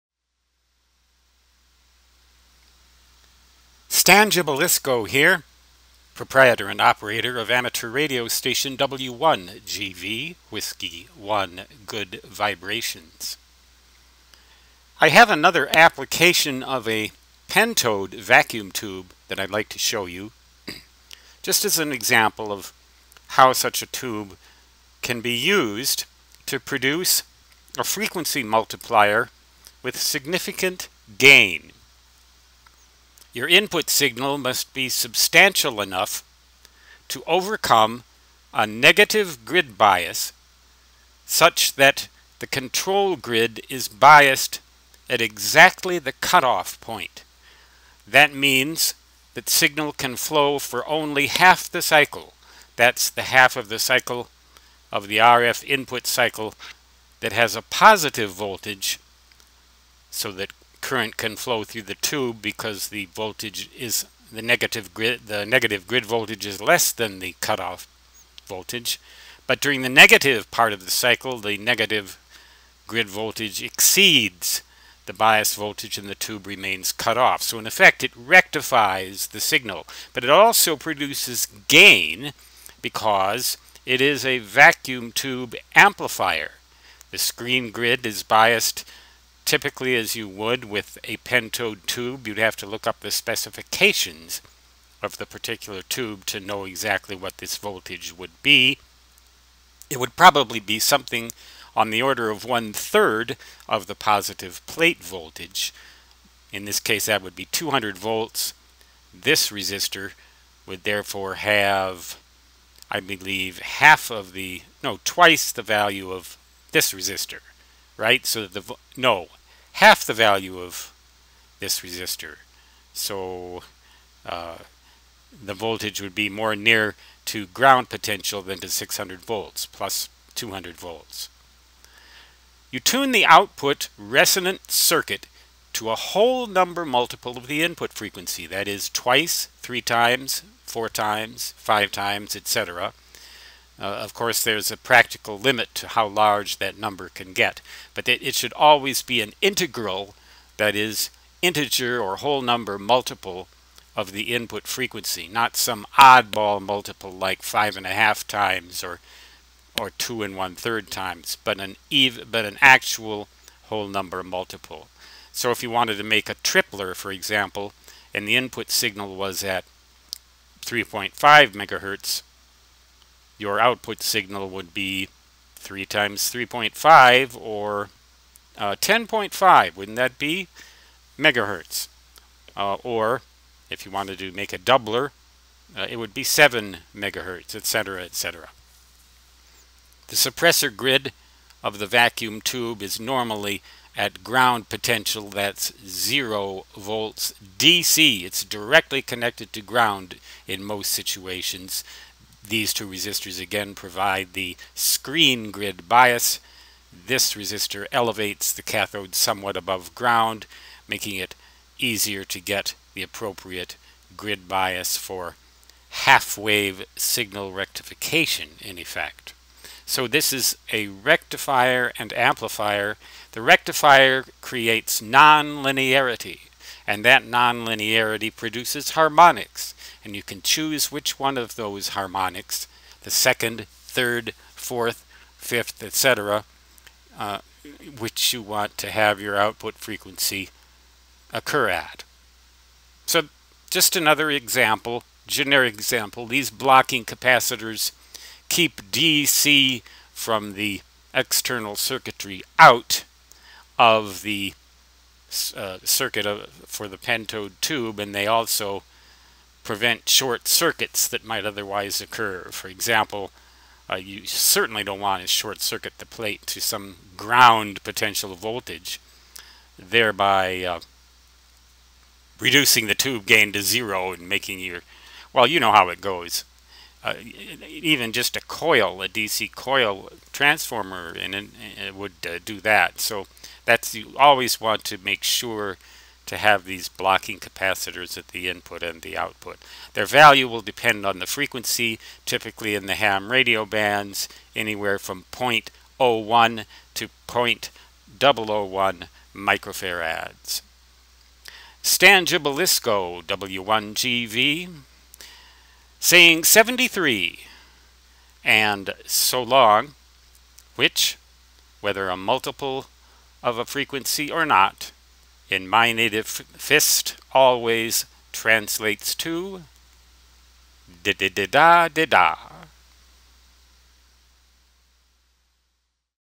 Pentode Frequency Multiplier
Here's an example of how a pentode vacuum tube can serve as a frequency multiplier and amplifier in the HF ham radio bands. 73 de W1GV.
sciencewriter-dot-net